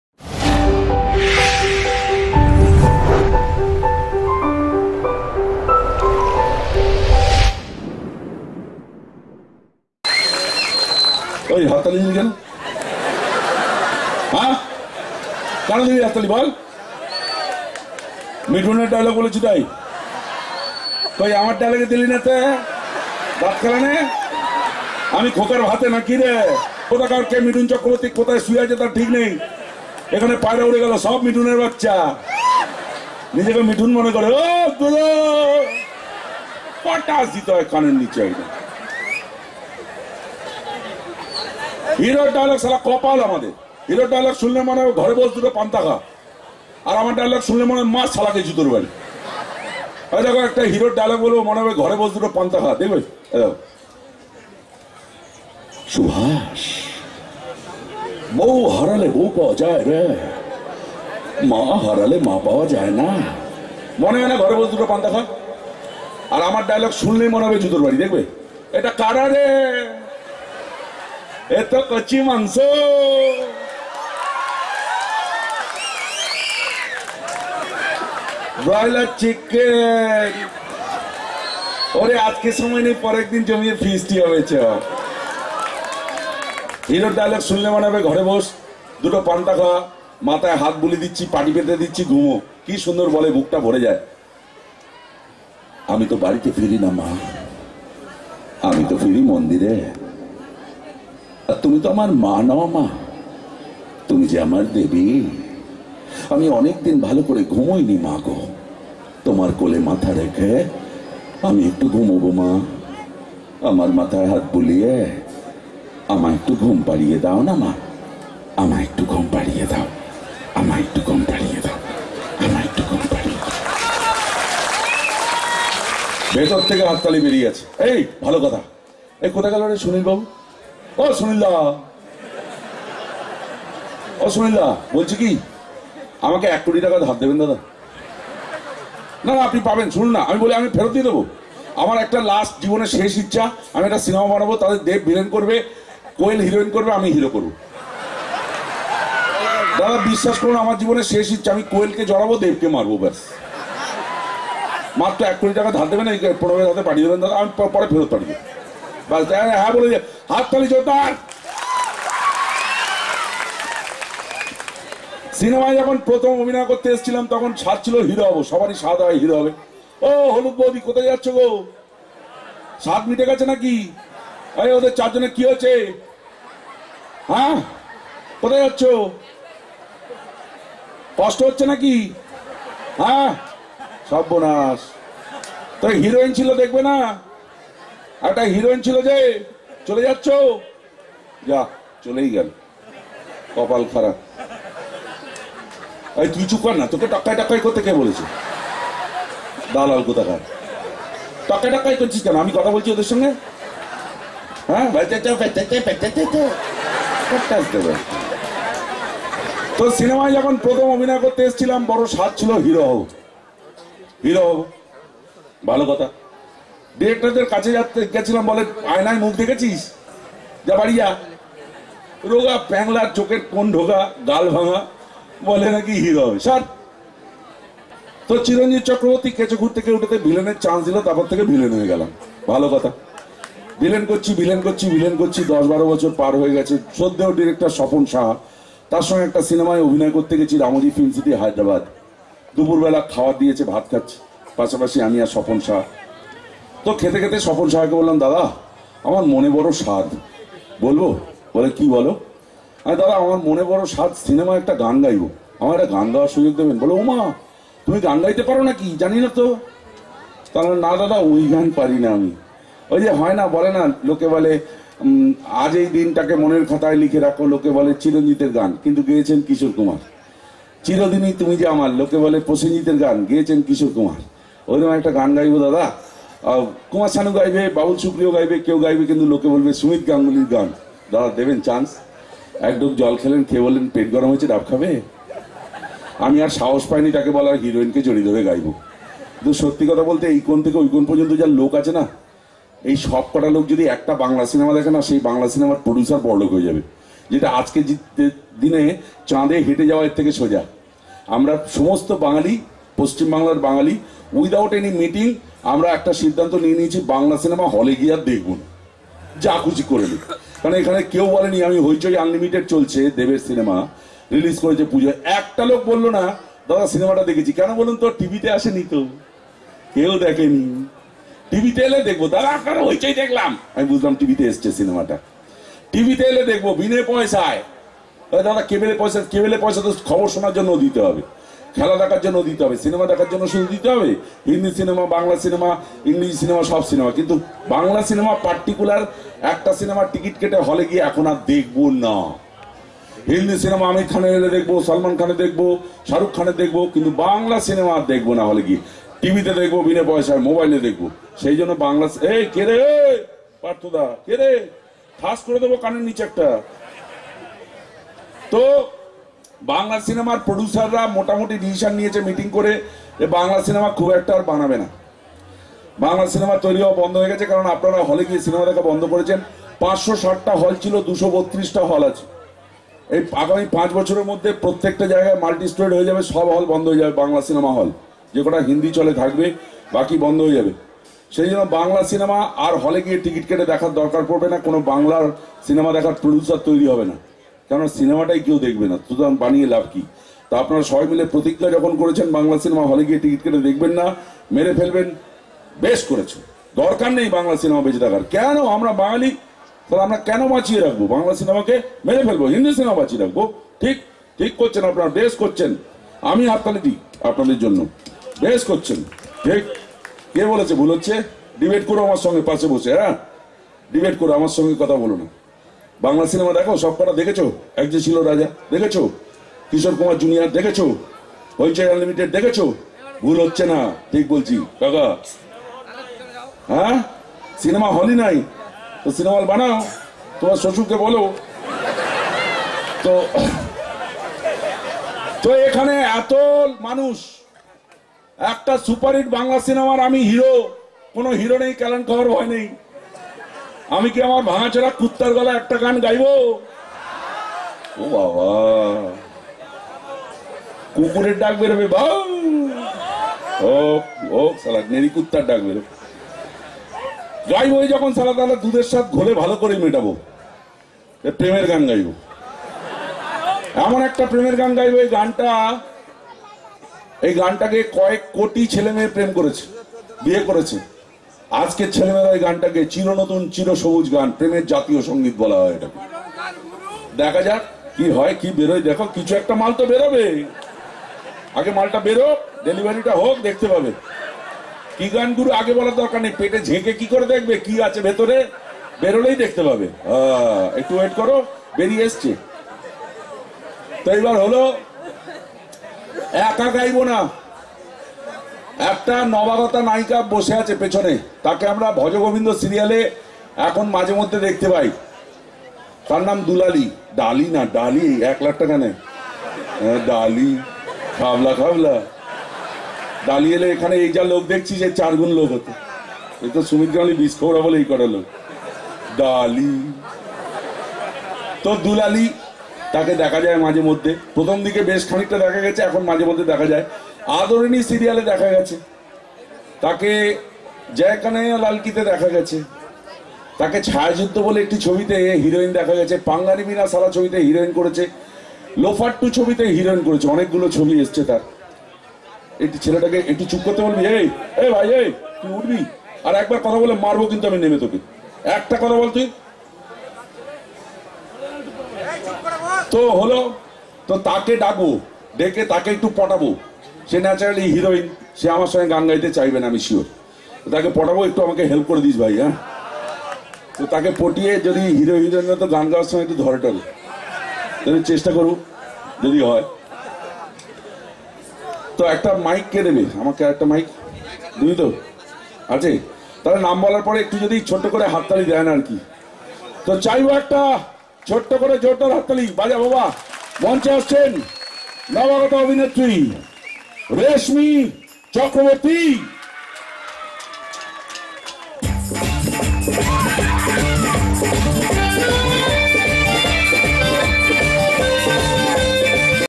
Stage programme/Sumit Ganguly/Comedy/Practical
বেলেপাড়া সার্বজনীন দুর্গোৎসবে অভিনেতা সুমিত গাঙ্গুলি ও অভিনেত্রী রেশমি চক্রবর্তী -এর সংগীতানুষ্ঠান।
Camera - Prasenjit